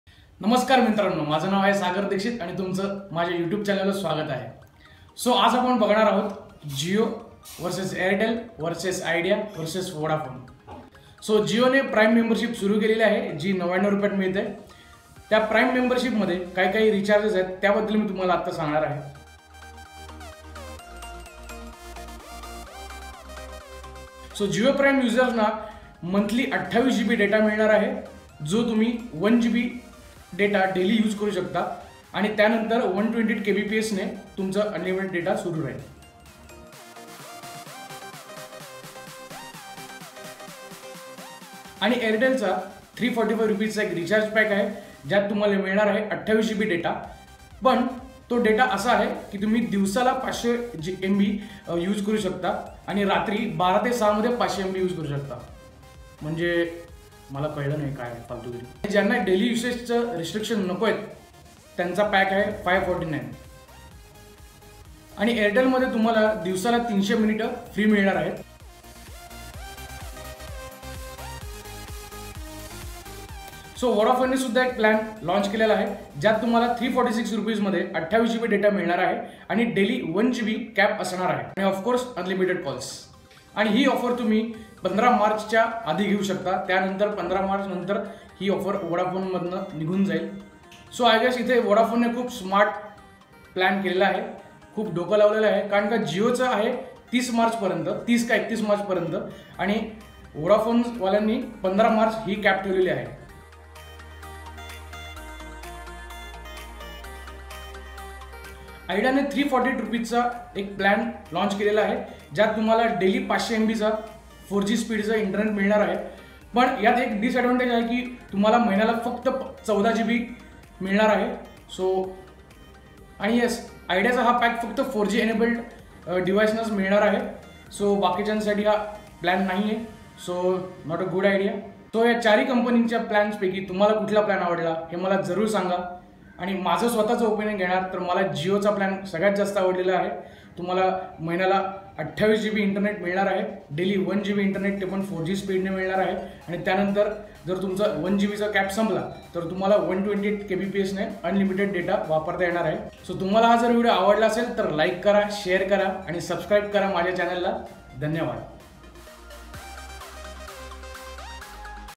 Jio vs Airtel vs Vodafone vs Idea !!! |Tug of War 2017 | New Unlimited Plans Explained [Marathi]
Today's video is about competition between jio, airtel, idea and vodafone.

country's top most telecom service providers comes face to face for their new unlimited services plan.

My vote goes for jio as i am very much happy for the internet data service. but still their calling services is not up to the mark (honestly it sucks !!! 5 calls try = 1 successful call .).
Vodafone is also offering great pack.

So guys tell me what will be your choice from these !!!

Please Subsribe To Channel !!!
Need Support From You Guys !!!